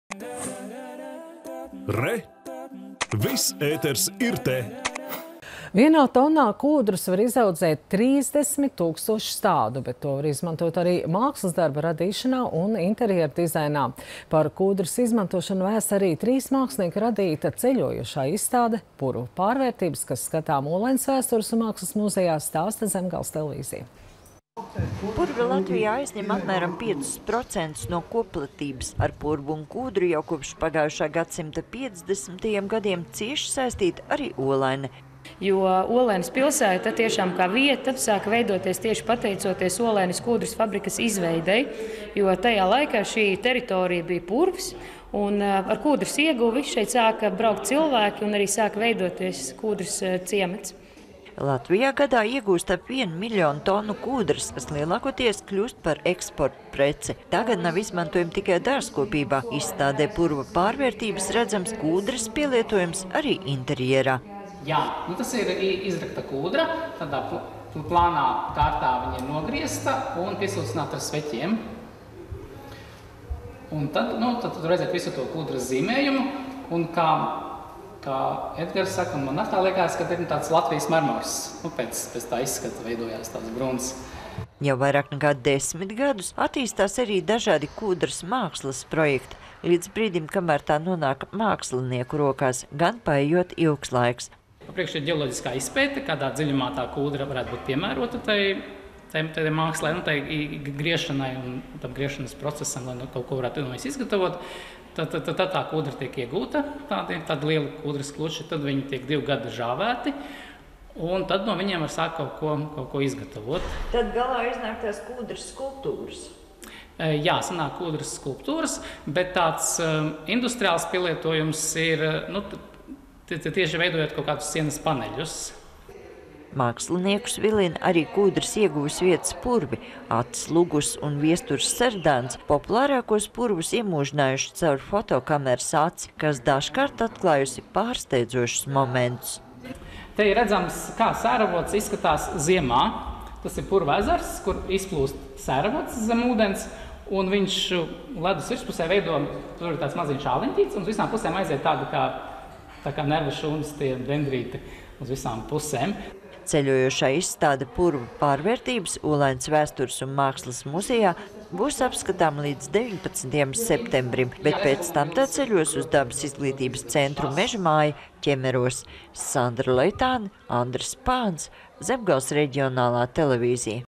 Olainē izstāde “Purva pārvērtības”
Vienā tonnā kūdras var izaudzēt 30 000 stādu, bet to var izmantot arī mākslas darbu radīšanā un interjera dizainā. Par kūdras izmantošanu vēsta arī  trīs mākslinieku radītā ceļojošā izstāde “Purva pārvērtības”, kas skatāma Olaines vēstures un mākslas muzejā. Par to – Zemgales TV sižetā.